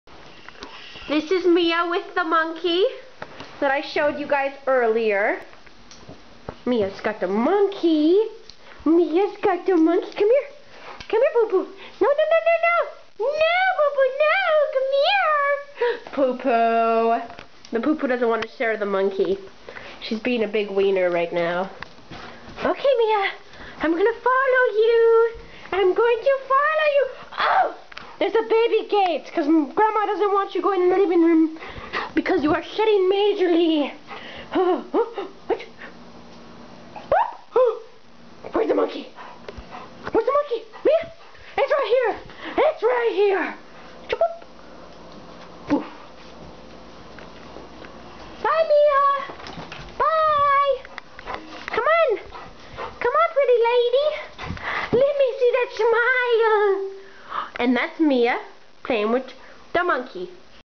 Mia, the Saint Bernard, with her monkey
This Mia and she is yet again playing with her monkey.